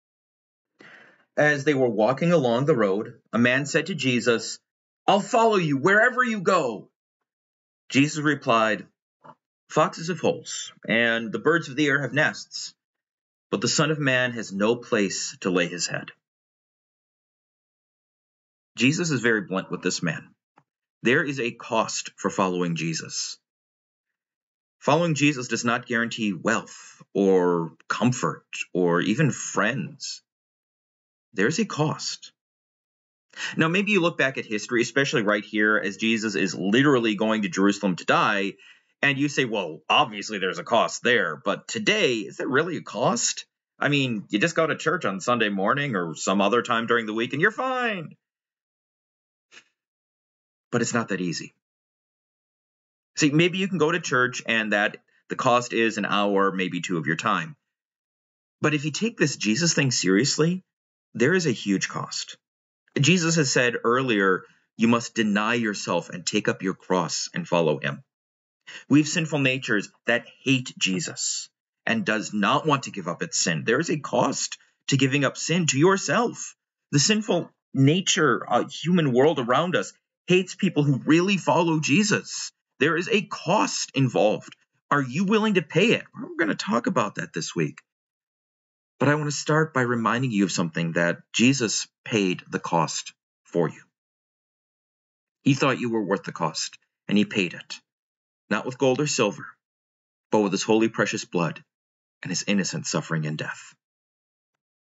There is a Cost
The man just wanted to follow Jesus. But Jesus is blunt. Following him isn’t easy. But… wait a second. It seems pretty easy these days, doesn’t it? Just go to church. But it means denying the self. We have sinful natures that want to rip us away from Jesus. And the sinful world doesn’t want us actually following Jesus. There’s a cost. Is it worth it? Jesus thought the cost was worth it to save you.